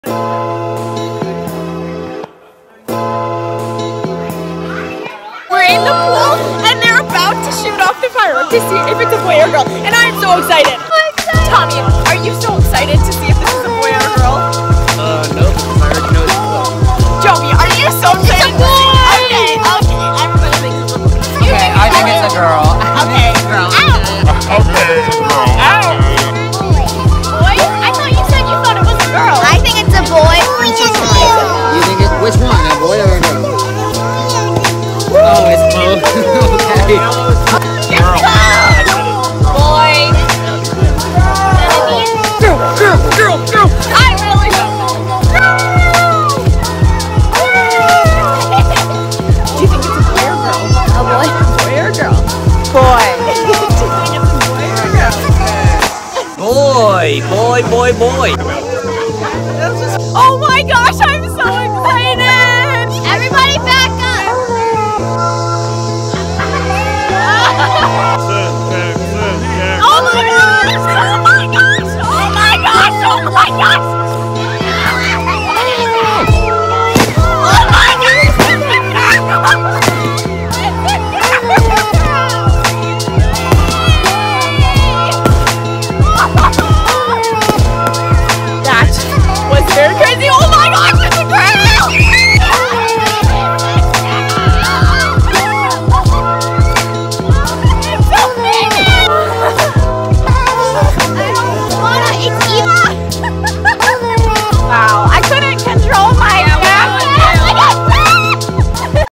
EPIC Gender Reveal for 6th child! What will the gender be?!?!
Watch the video to find out what gender the 6th child will be! This was an awesome gender reveal party!!!